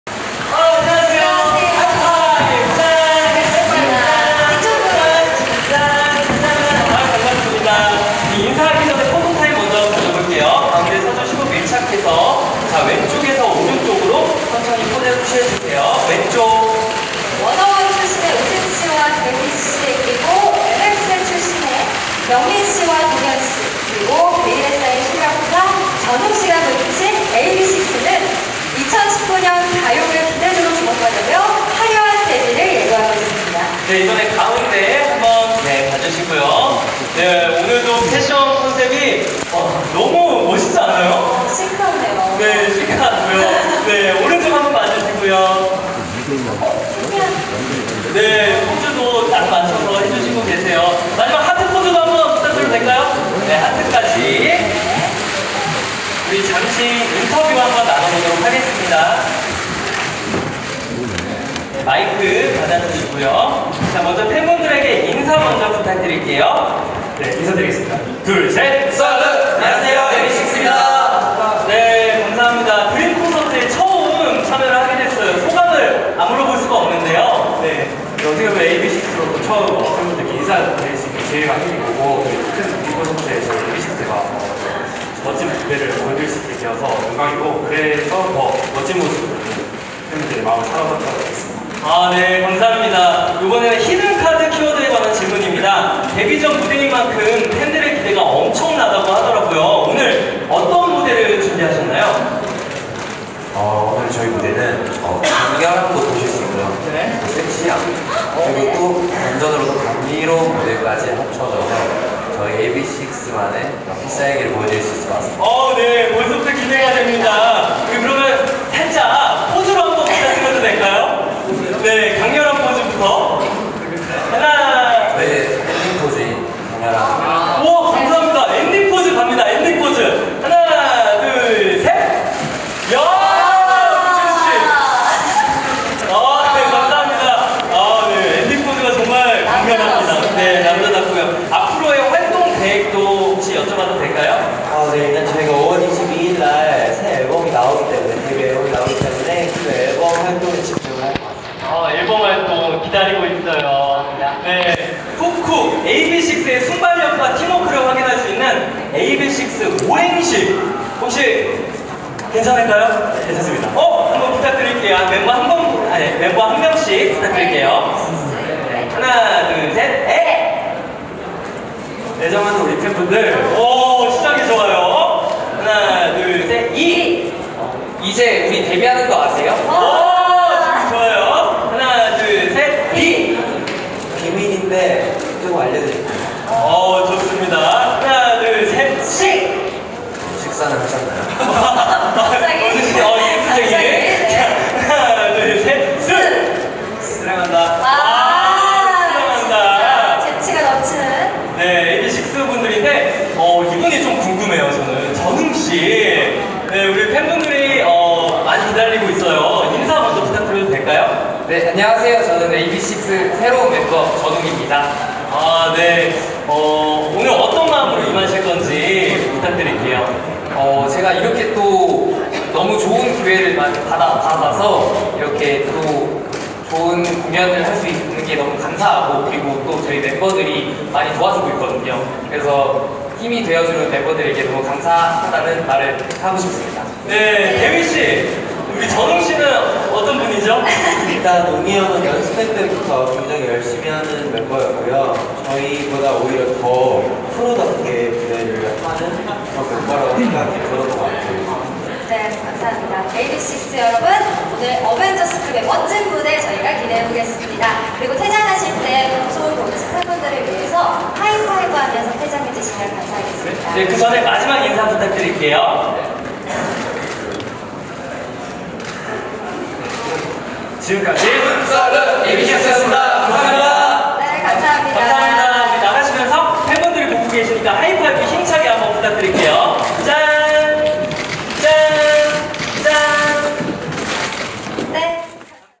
에이비식스 2019 드림콘서트 AB6IX @ the 25th DREAM CONCERT
18일 오후 서울 상암 월드컵경기장에서 제25회 2019 드림콘서트가 5만여명의 관객의 열띤 호응속에 개최됐다. AB6IX가 레드카펫에 참석해 포토타임을 갖고 있다.